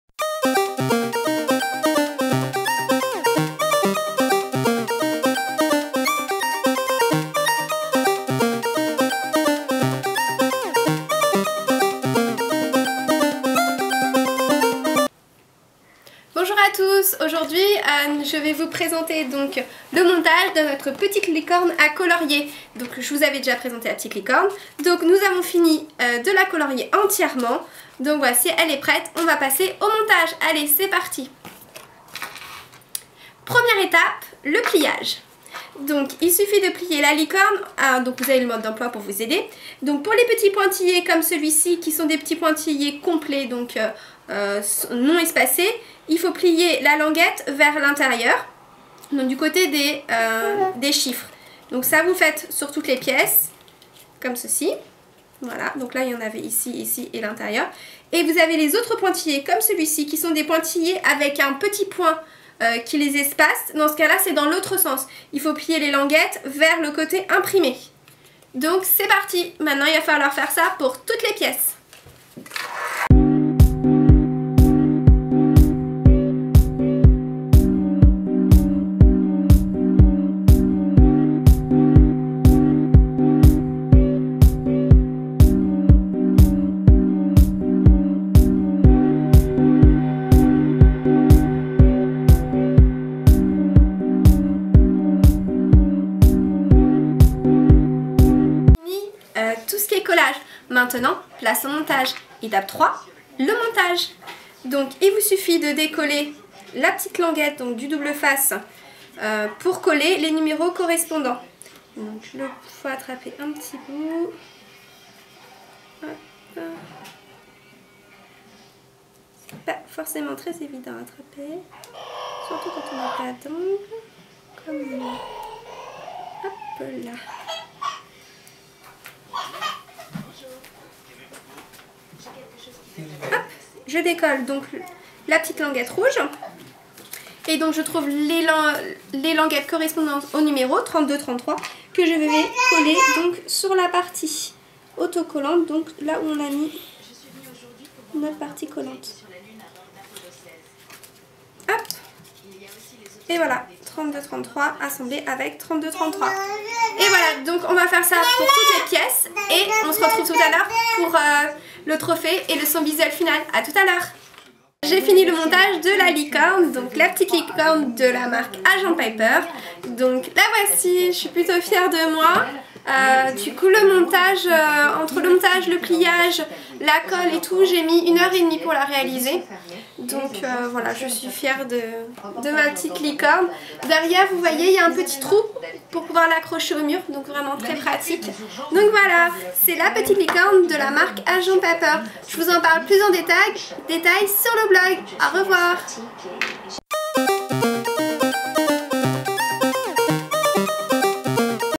montage petite licorne agent paper
Je vous montre en vidéo comment monter votre petite licorne à colorier de la marque Agent Paper.